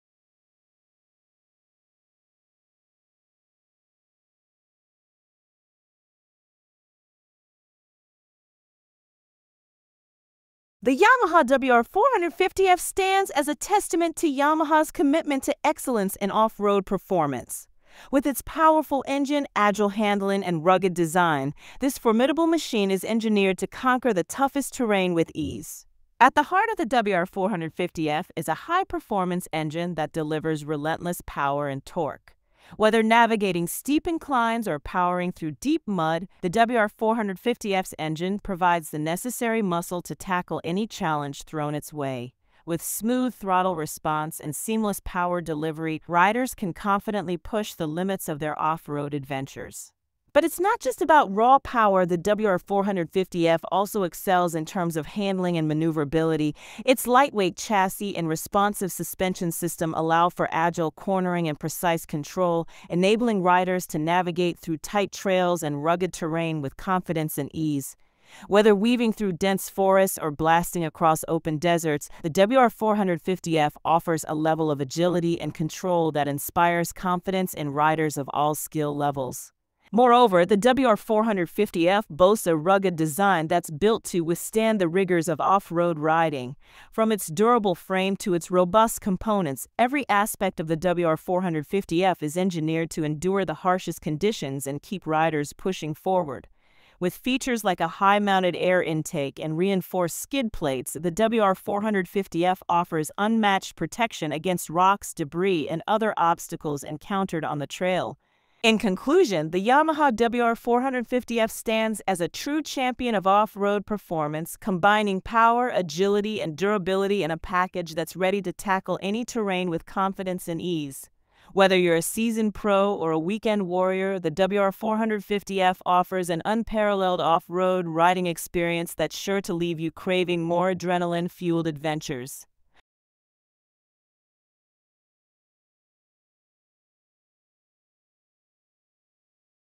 CONQUERING TERRAIN EXPLORING THE YAMAHA WR450F'S DOMINANCE OFF ROAD
At the heart of the WR450F is a high-performance engine that delivers relentless power and torque. Whether navigating steep inclines or powering through deep mud, the WR450F's engine provides the necessary muscle to tackle any challenge thrown its way. With smooth throttle response and seamless power delivery, riders can confidently push the limits of their off-road adventures.